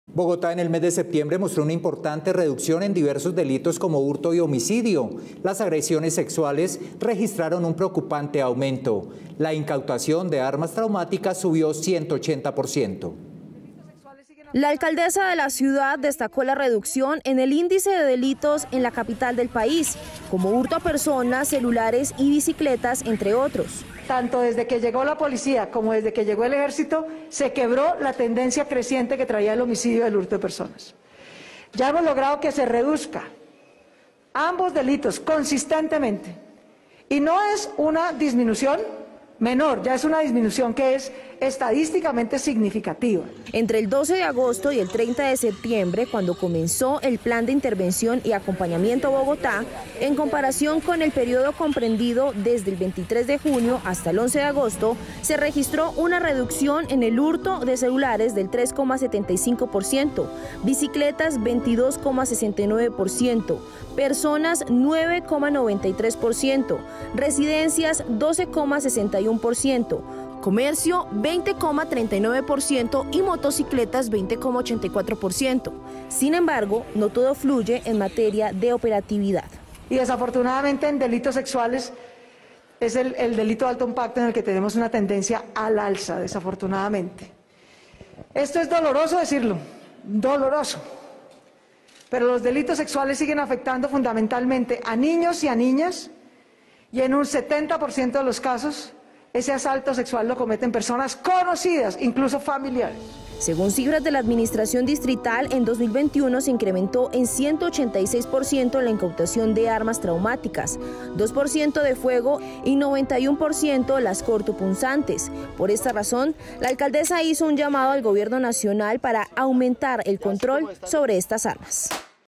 Bogotá tuvo reducción de algunos delitos en septiembre del 2021
NoticiasCapital | Bogotá registró la disminución en casos de hurto y homicidio en septiembre. La alcaldesa explicó estos datos estadísticos relacionados con la seguridad de la capital.